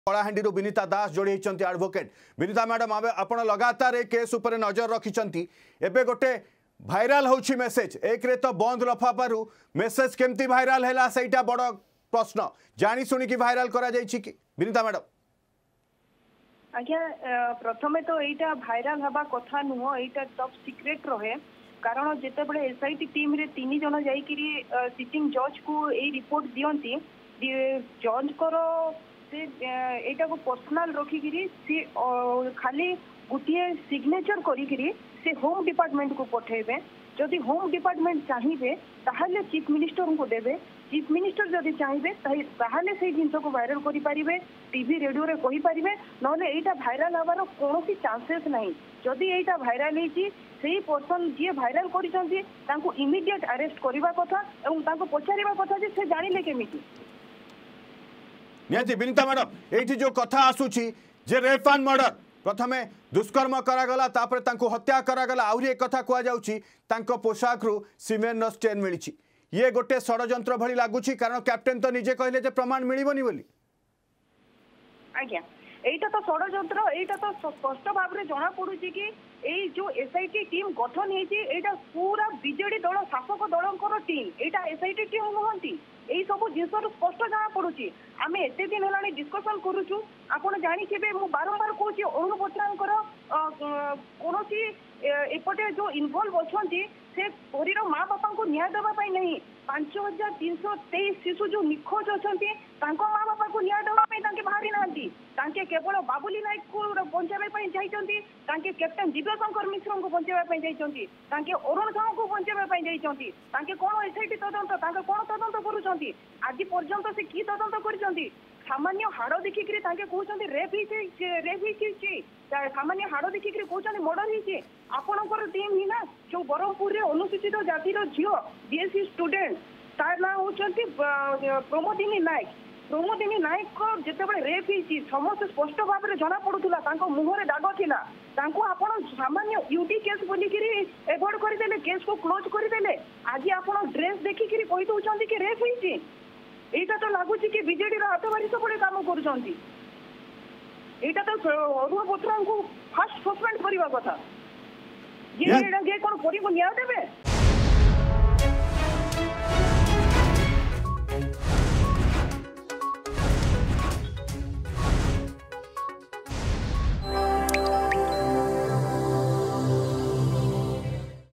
ବନ୍ଦ ଲଫାଫାରୁ ମେସେଜ ଭାଇରାଲ୍
| Justice for Paree ବନ୍ଦ ଲଫାଫାରୁ ମେସେଜ ଭାଇରାଲ ହେବା ଏକ ଅପରାଧୀକ କାର୍ଯ୍ୟ। ଯେଉଁମାନେ ଏ ଲଫାଫାକୁ ଭାଇରାଲ କରିଛନ୍ତି ସେମାନଙ୍କ ବିରୋଧରେ ଦୃଢ କାର୍ଯ୍ୟାନୁଷ୍ଠାନ ଗ୍ରହଣ କରାଯାଉ ବୋଲି ଦାବି ରଖିଛନ୍ତି ଆଇନଜୀବି ବିନିତା ଦାସ.